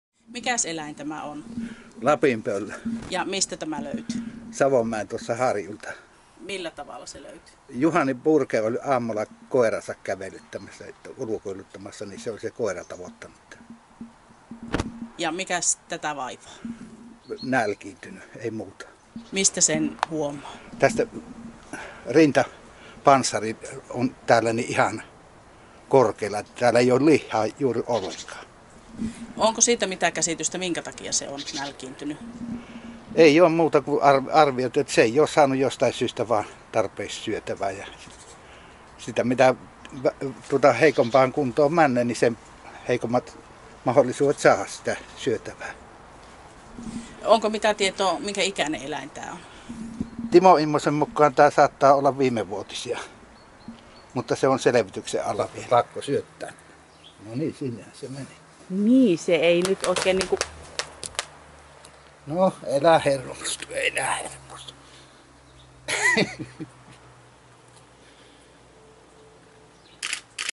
lapinpöllö
Varkautelainen Markku Korhonen hoitaa nälkiintyneenä löytynyttä lapinpöllöä. Pöllö on tarkoitus päästää takaisin vapauteen heti, kun linnun kunto sen sallii.